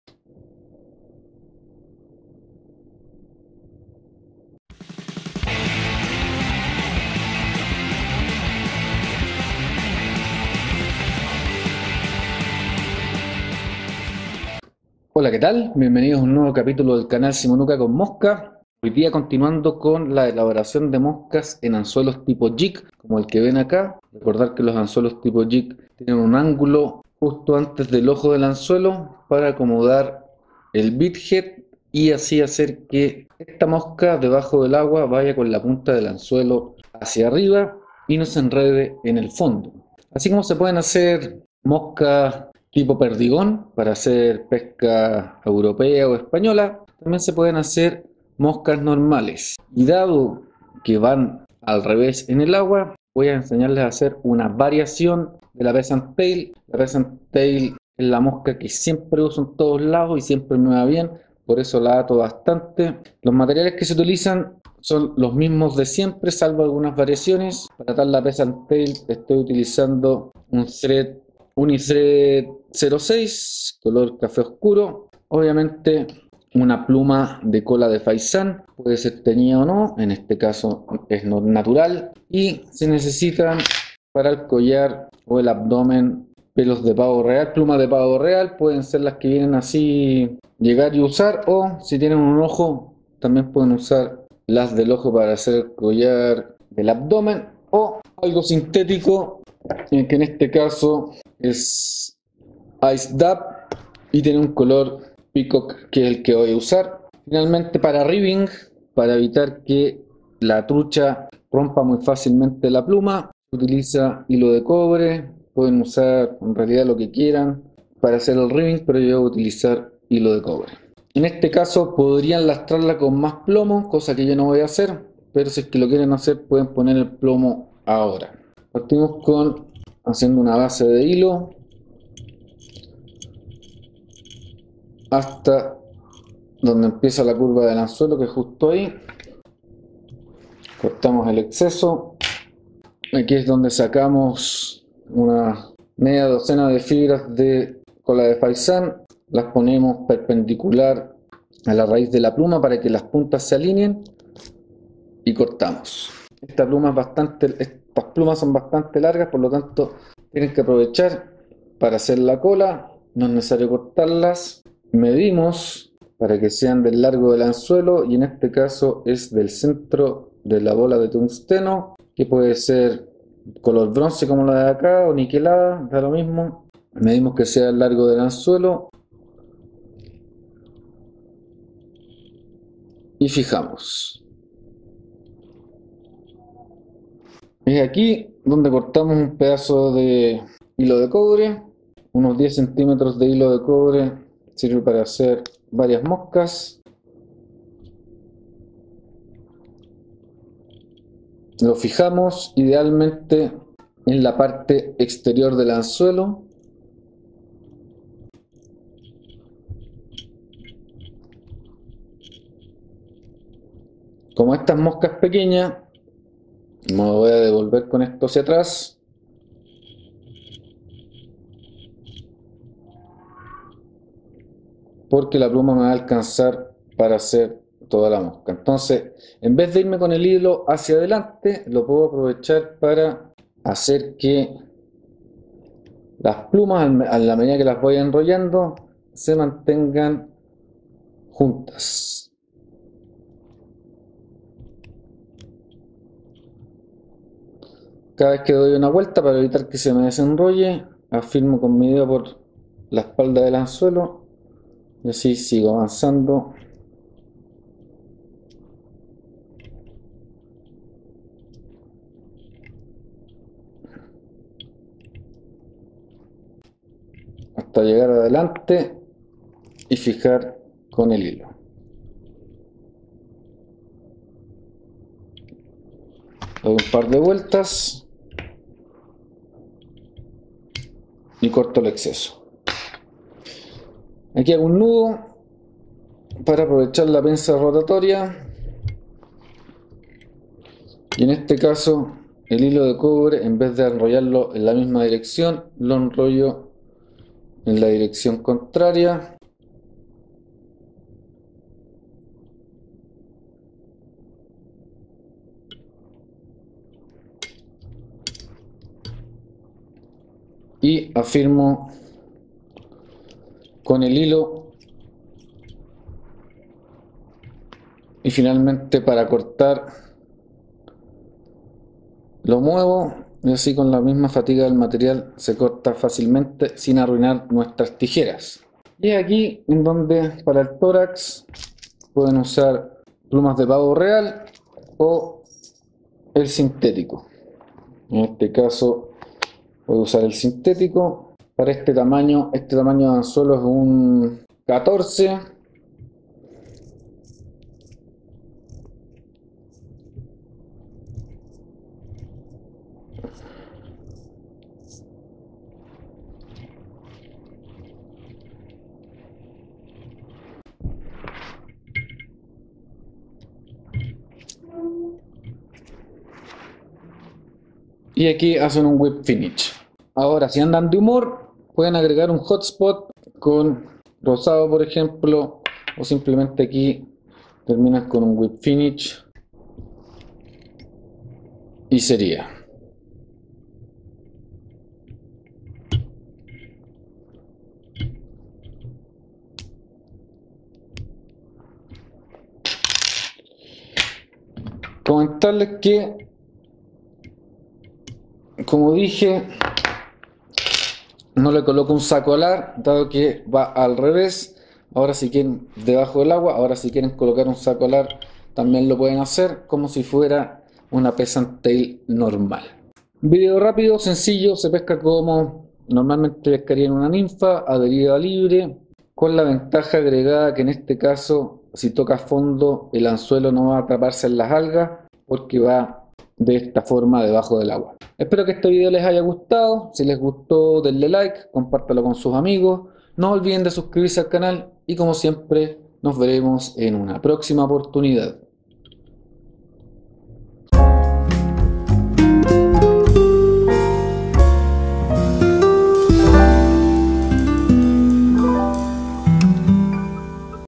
Montaje (atado) de una Pheasant Tail (ninfa) en anzuelo jig para la pesca con mosca
Atando una pheasant tail en anzuelo jig. Ésta es una ninfa muy versátil, del tipo atractora, que me ha dado buenos resultados en muchos lados, tanto en Chile como en el extranjero.
En éste caso en vez de usar un anzuelo tradicional, utilizo un anzuelo tipo "Jig" para que la punta del anzuelo valla invertida y no se enrede tán fácilmente en el fondo. Además hago un par de variaciones, pero se puede atar el patrón de siempre en el anzuelo, haciendo el saco alar tanto por arriba como por abajo.

Anzuelo: Tipo Jig
Peso: Bola de tungsteno
Cola: 6 fibras de cola de faisan.
Cuerpo: seis fibras de cola de faisan.
Rib: Alambre de cobre.
Torax: Dubing color pavo real.
Hilo: 6/0 or 8/0. Usualmente negro o cafe.